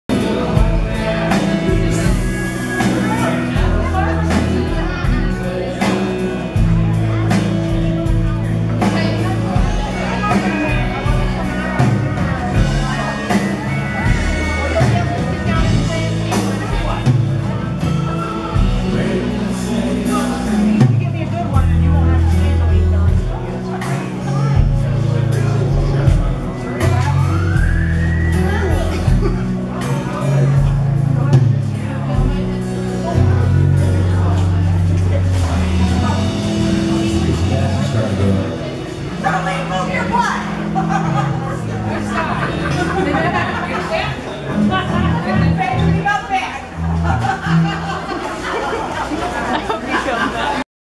Joe Nichols Sound Check 3
Sound Check at the Ashtabula County Fair, Ohio. August 8, 2013